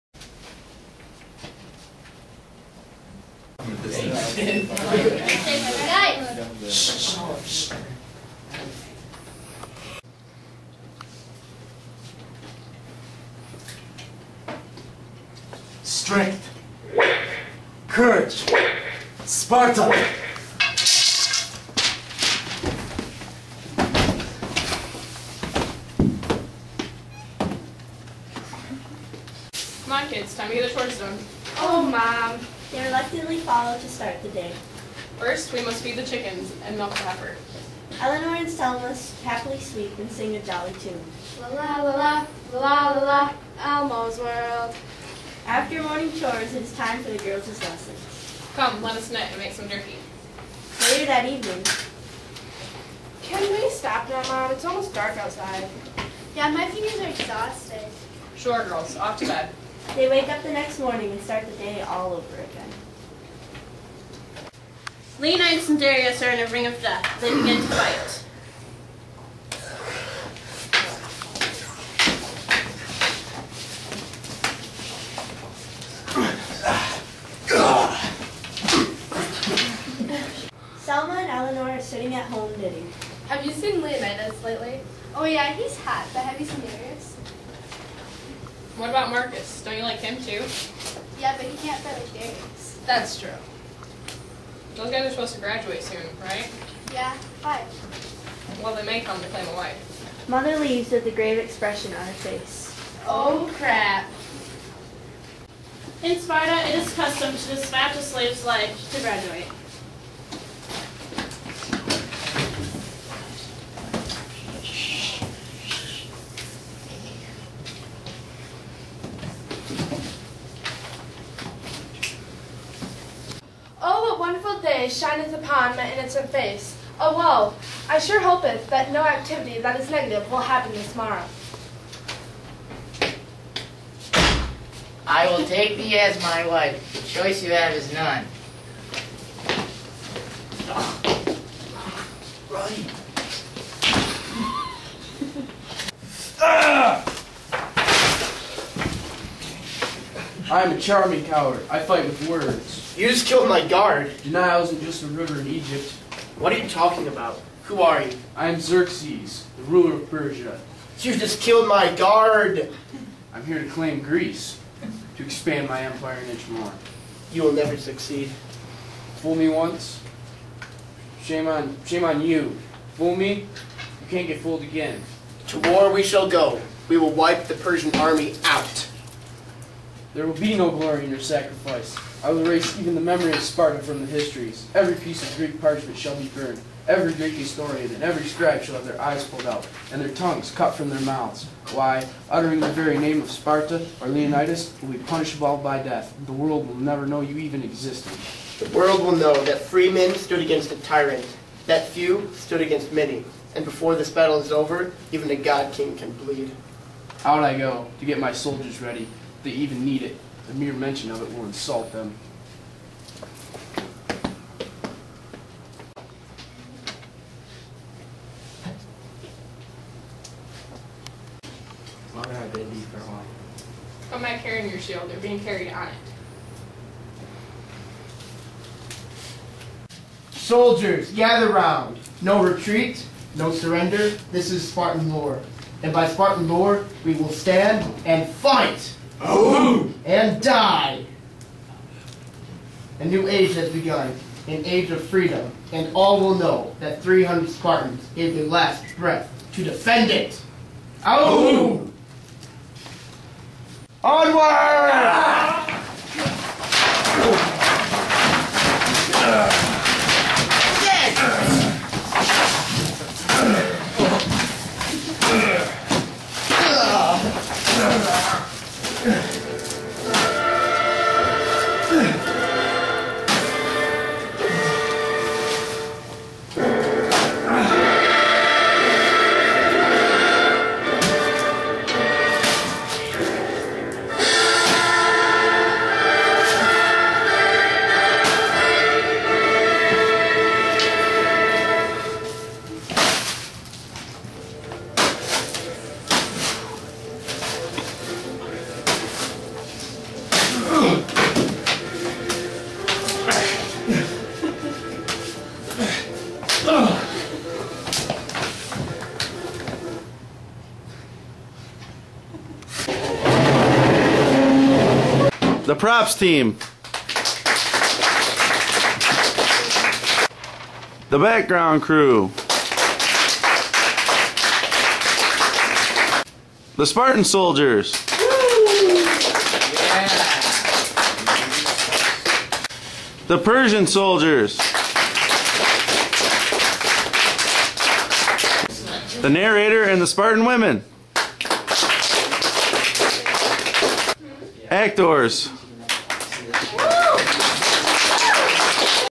Drag Me To Persia (Durand High School)
Student-produced movie about the life and times of men and women in Ancient Greece.

Mr. Inman's World History 1 class.

2nd trimester, 1st hour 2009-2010 year.